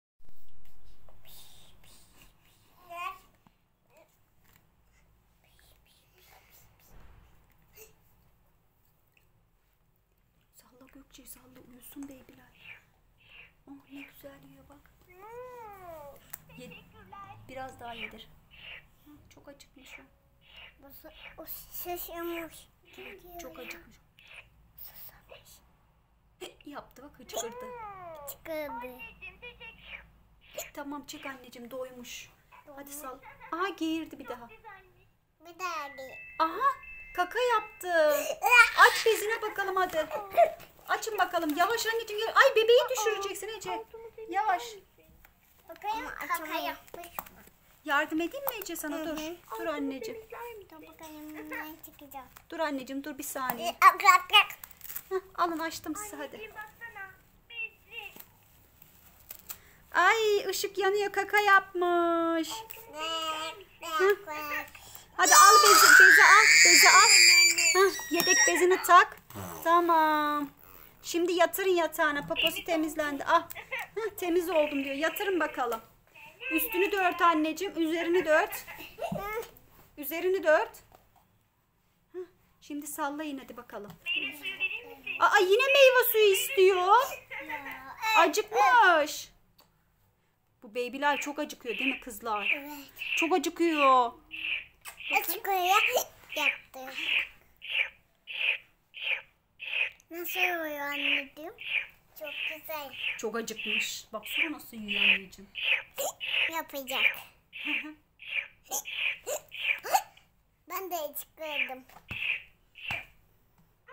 Ece Gökçe Bebek Bakma Oyunu Oynuyor Baby Alive yi Uyutuyorlar.
Ece Gökçe Playing Baby Care Game They Sleep Baby Alive